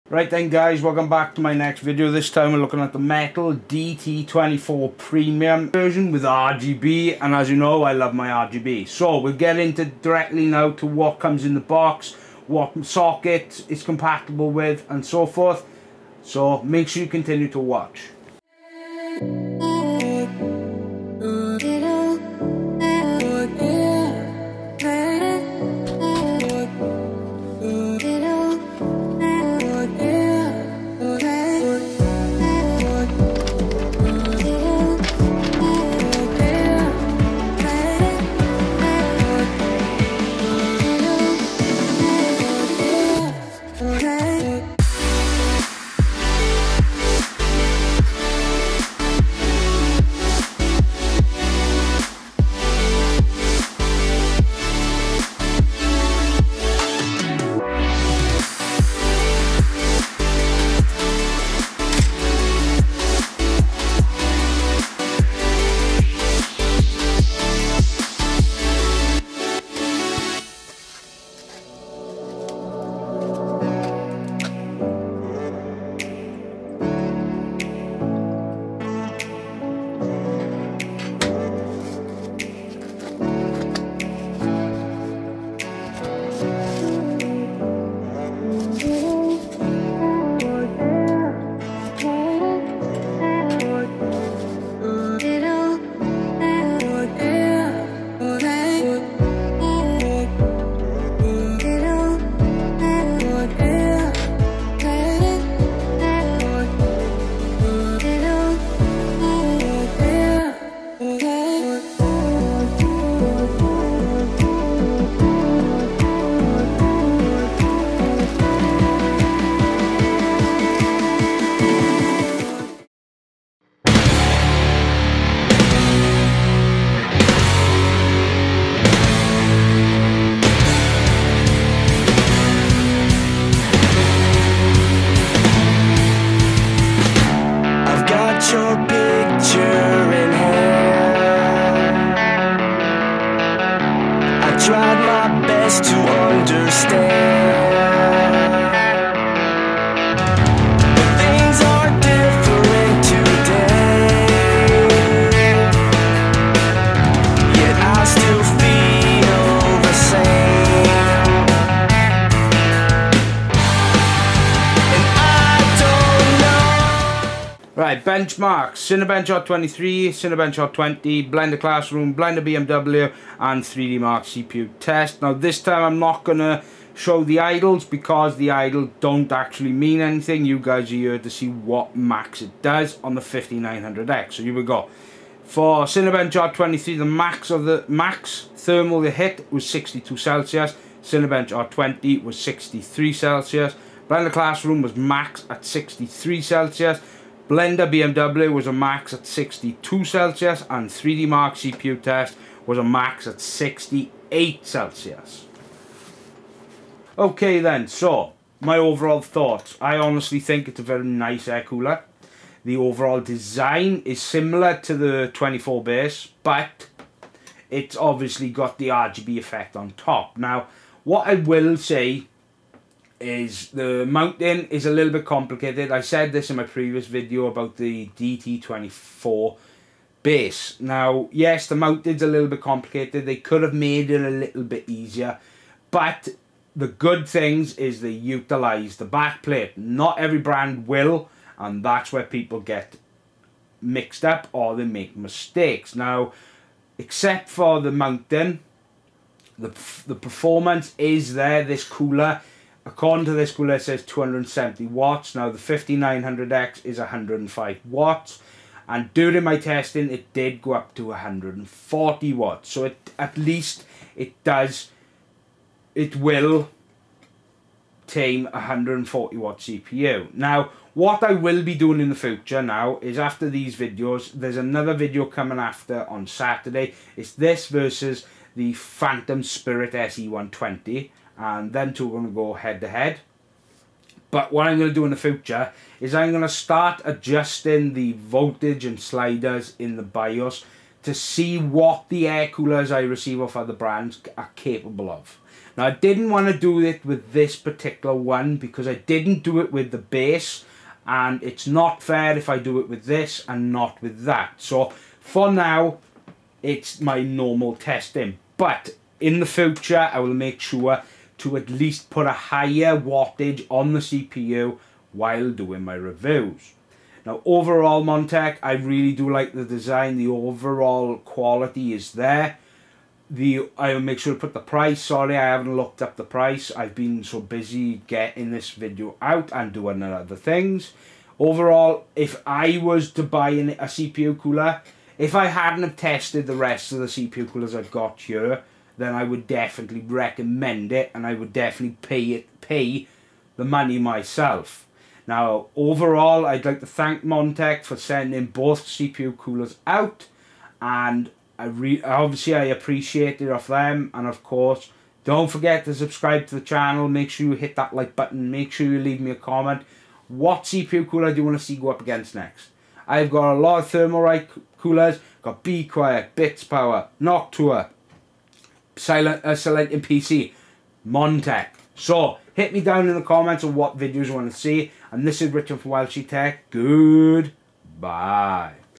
Best Cpu Cooler ? Montech Metal DT24 Premium Cpu Cooler Review
is the Montech Metal DT24 Premium really the best Cpu Cooler well today we will test it on the AMD Ryzen 9 5900x CPU and see if its the best
00:00 Introduction
00:19 Coming Saturday
00:30 What's included in the box
02:03 B-Roll What it looks like Powered on
02:48 Thermal Results 5900X
03:31 My overall Thoughts
04:41 More content coming
05:28 Price from Amazon & CCL
05:43 Don't forget to Subscribe